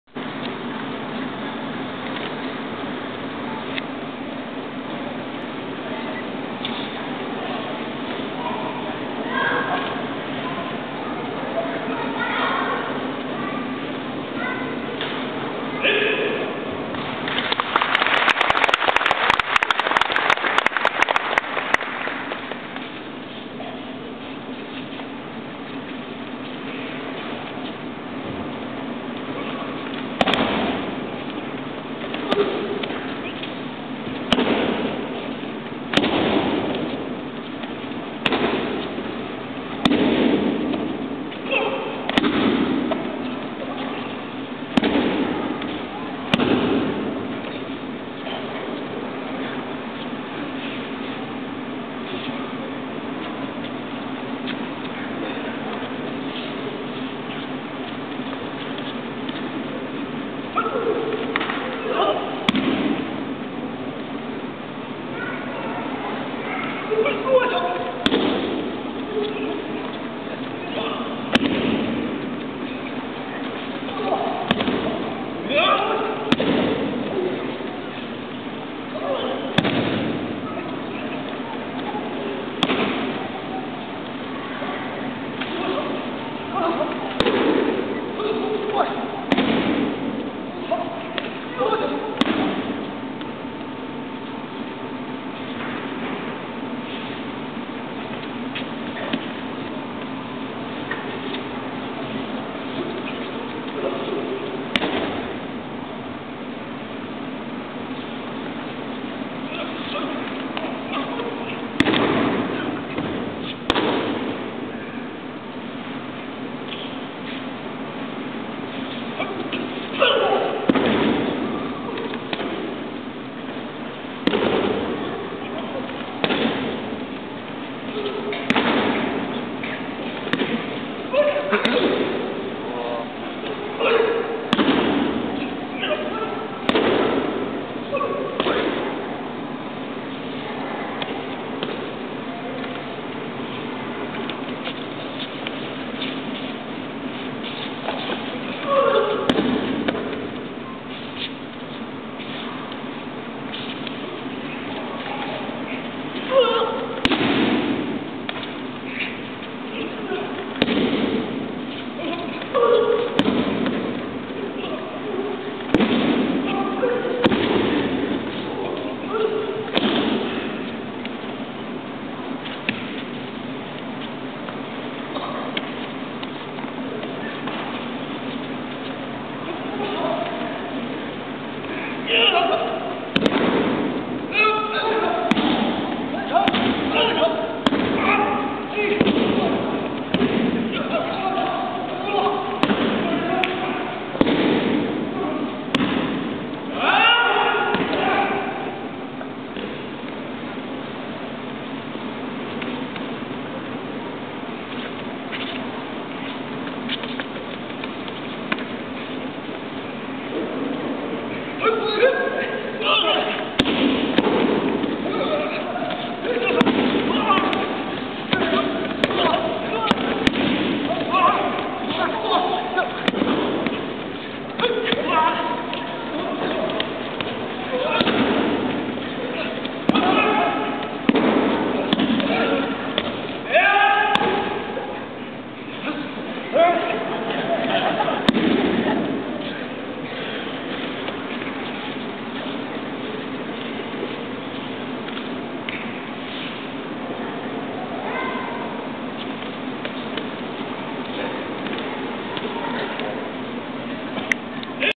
Ando Shihan 8th Dan Yoshinkan
Yoshinkan Aikido Gozo Shioda Memorial Festival Japan 2010 Ando Shihan 8th Dan Yoshinkan, Ryu Dojo.